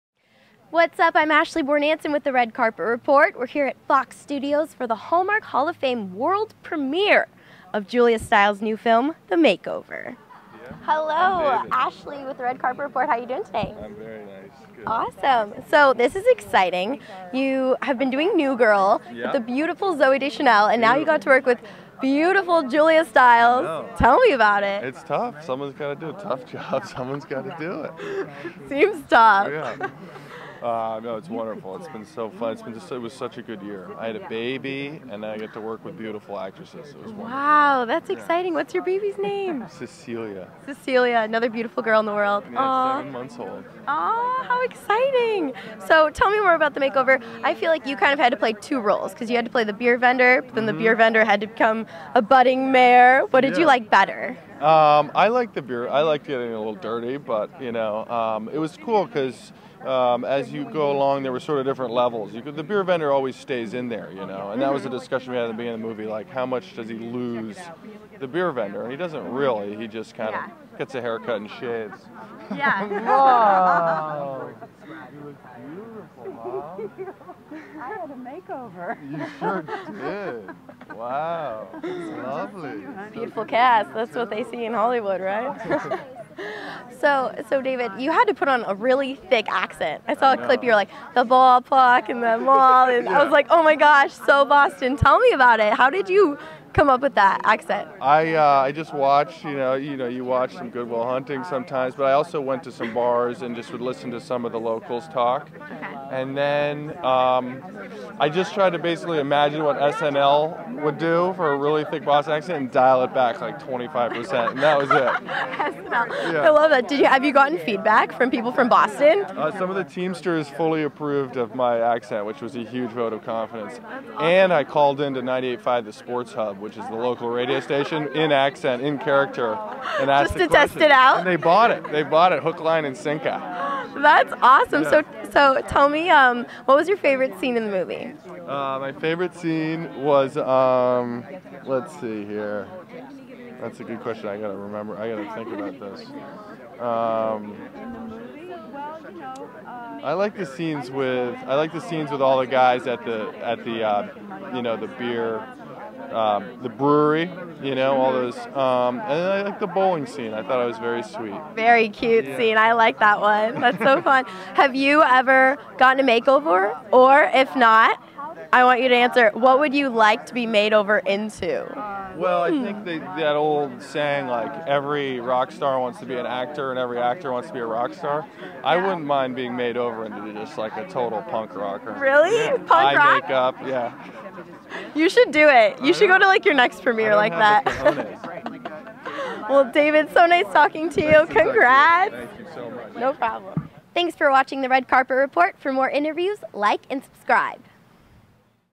David Walton at Hallmark Hall of Fame's "The Makeover" Red Carpet
Mingle Media TV and Red Carpet Report host, Ashley Bornancin, were invited to cover the red carpet for the Premiere of The Hallmark Hall of Fame's production of "The Makeover" starring Julia Stiles, David Walton and Camryn Manheim. "The Makeover" will premiere on Sunday, January 27 at 9:00 PM ET on the ABC Television Network.

About Hallmark Hall of Fame and "The Makeover"
Hallmark Hall of Fame has been creating original films designed to entertain, enlighten and inspire for over sixty years. Hannah Higgins (Julia Stiles) is a smart, driven Boston consultant. To advance the
cause of education reform, she runs for Congress -- but loses, big‐time. Voters just can't warm to her. For the next election she and her business partner, Colleen Pickering (Camryn Manheim), recruit beer salesman Elliot Doolittle (David Walton). He speaks with a thick South Boston accent, has an unruly beard, and dresses super‐informally.
Hannah takes Elliot on as a "project." She tries to get him to speak like a gentleman, dress like a gentleman, comport himself like a gentleman. As Elliot becomes more "presentable" and Hannah becomes less formal, could this turn in to a love match made in electoral Heaven?  Filmed on location in Boston, Massachusetts.
The movie was directed by John Gray ("Ghost Whisperer," "Empire") and the teleplay was written for "The Makeover" by C. Jay Cox ('Sweet Home Alabama," "Latter Days"). The film is produced by Christopher Morgan ("Common Law," "Memphis Beat"). Brent Shields ("A Smile as Big as the Moon," "November Christmas") is executive producer, for Hallmark Hall of Fame Productions, Inc.

by OneLoad.com